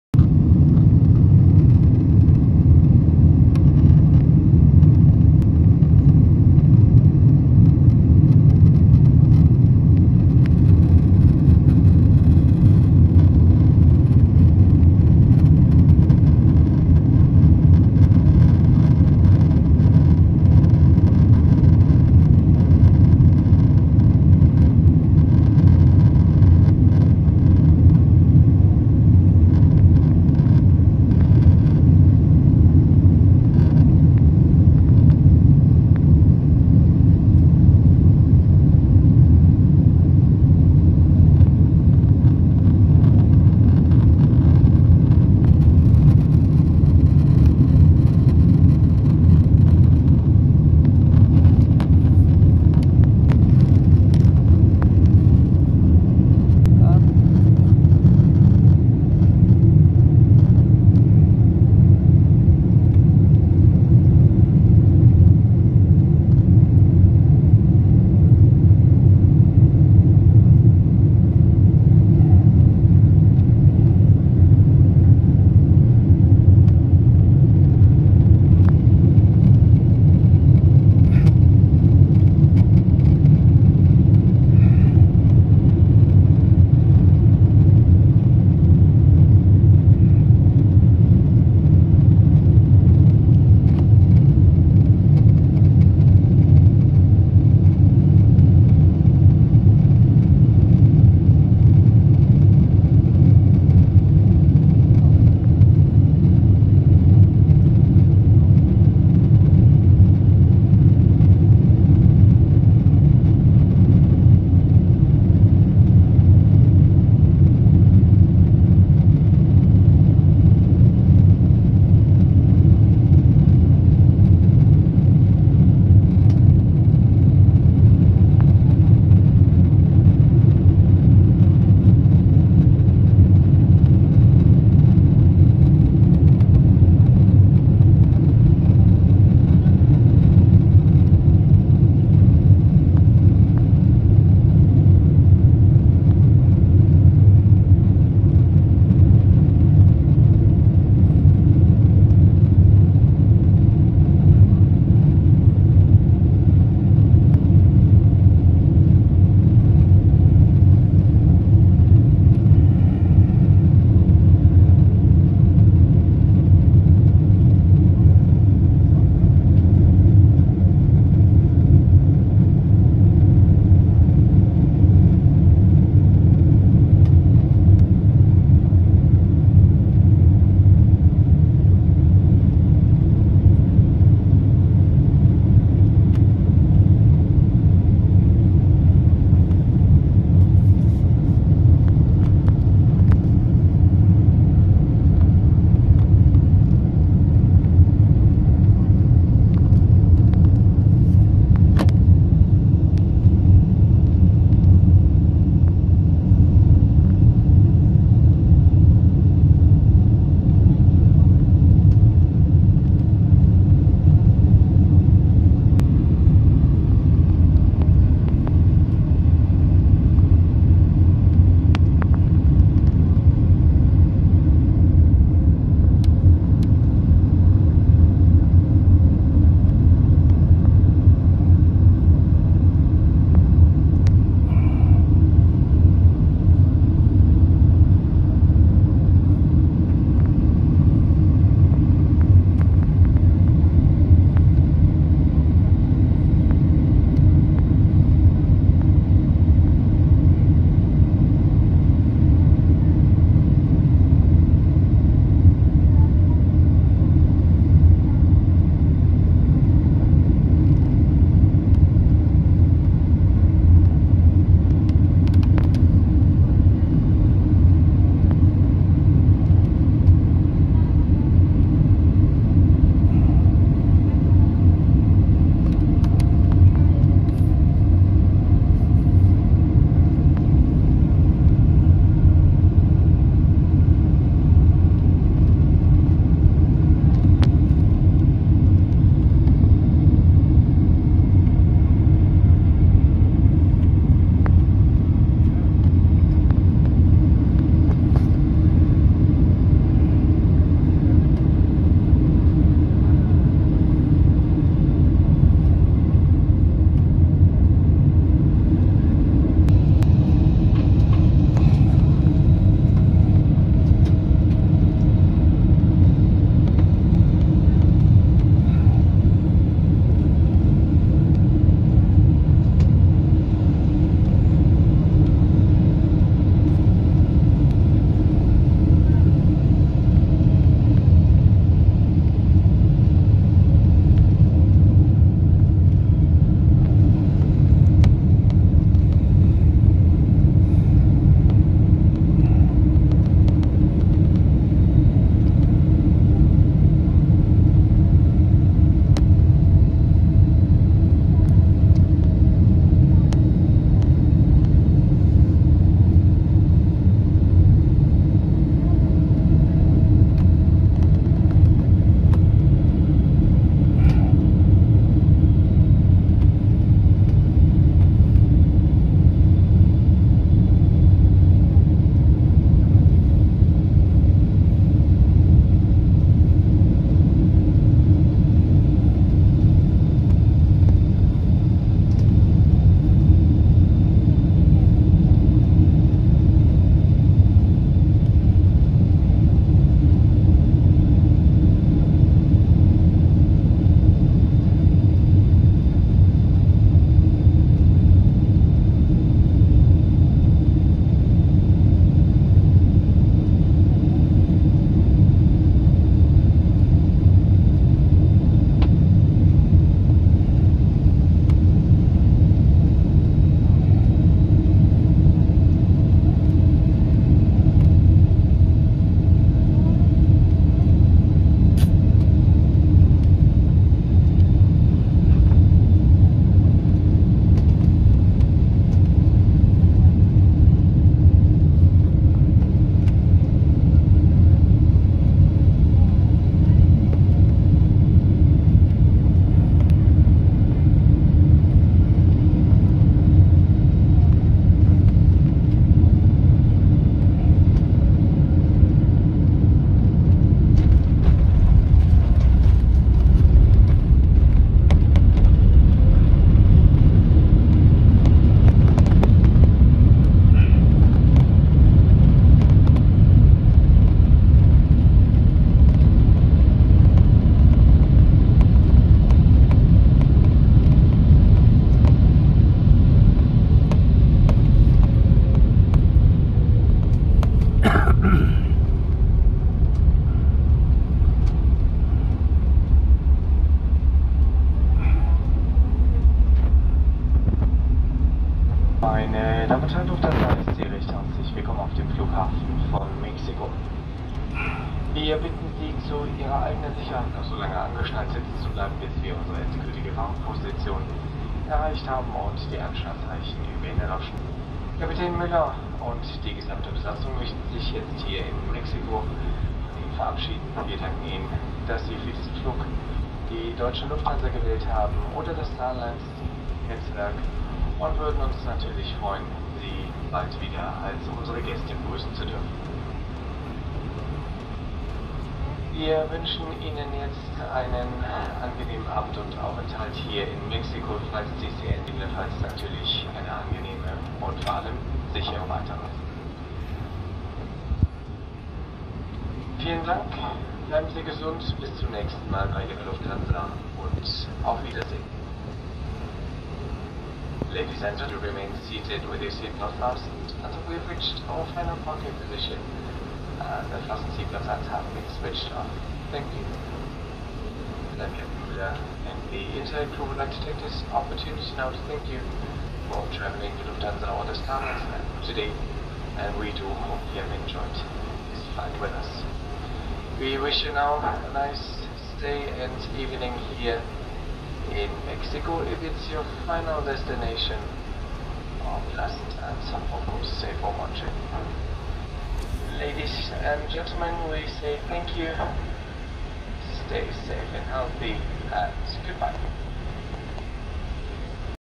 498 Frankfurt FRA 🇩🇪 Mexico MEX 🇲🇽 14.23-18.28 Amazing approach, landing into
seat: 86A Biz
The Majesty of Ciudad de Mexico lufthansa @AIMC_mx @airportfra
Very first flight from FRA to MEX post COVID19's outage